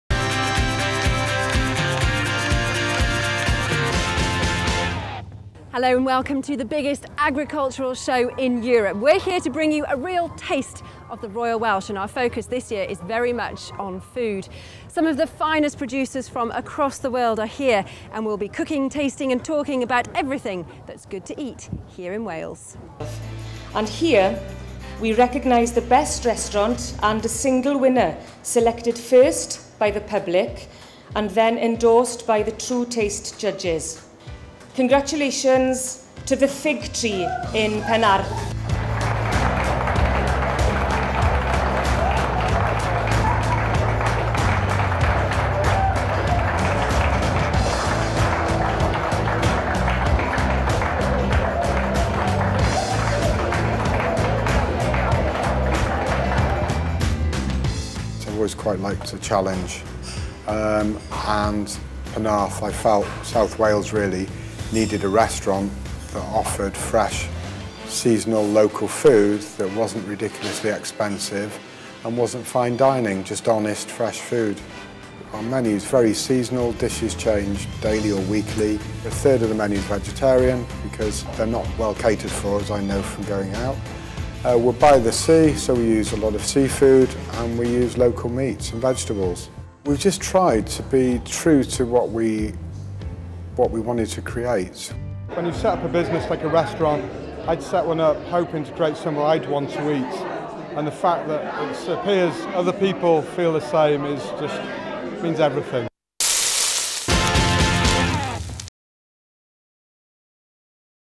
True Taste Award Presentation "Gold" goes to the Fig Tree Restaurant Penarth
True Taste Gold Award Presentation
Best Restaurant 2012
The Royal Welsh Show